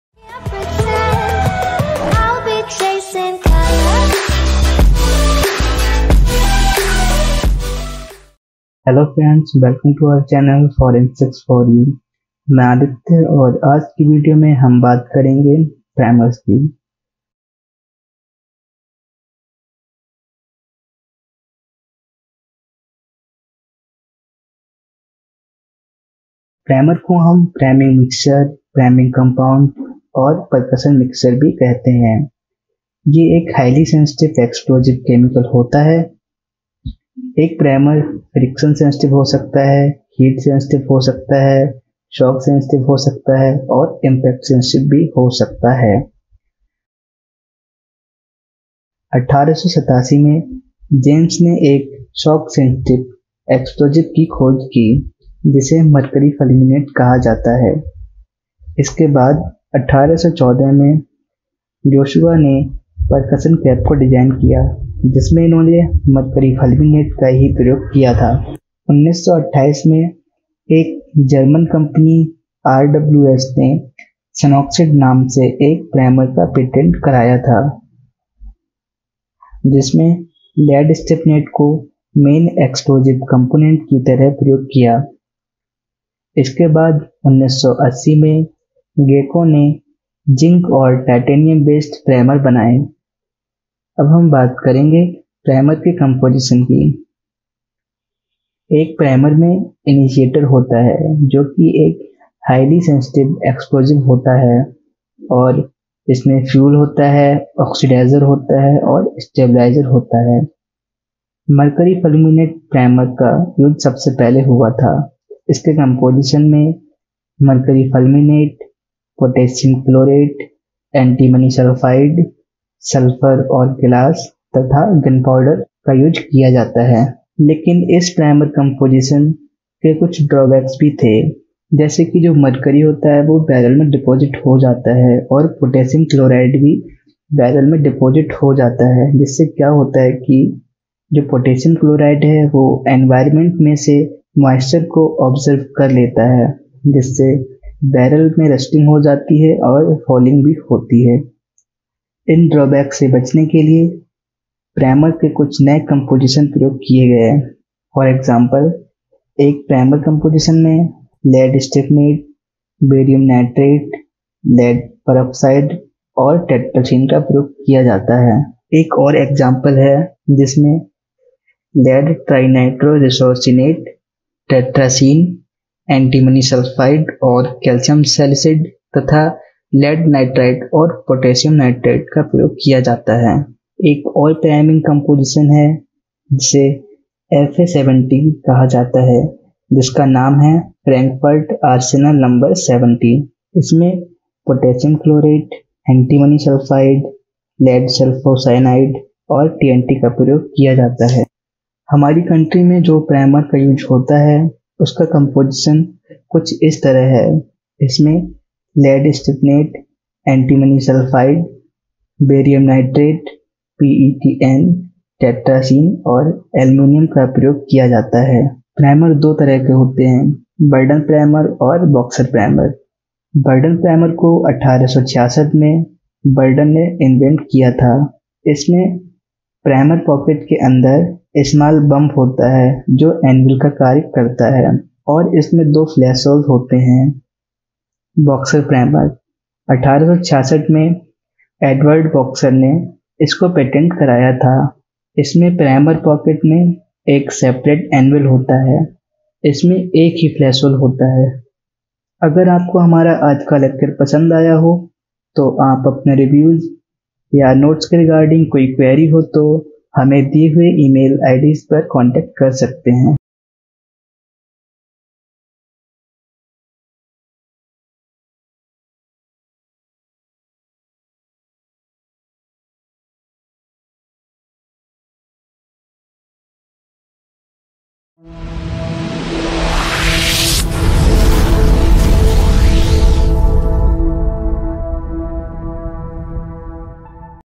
INTRODUCTION TO PRIMERS
This video has been made to provide information regarding the basics of primers essential for the field of Forensic Science.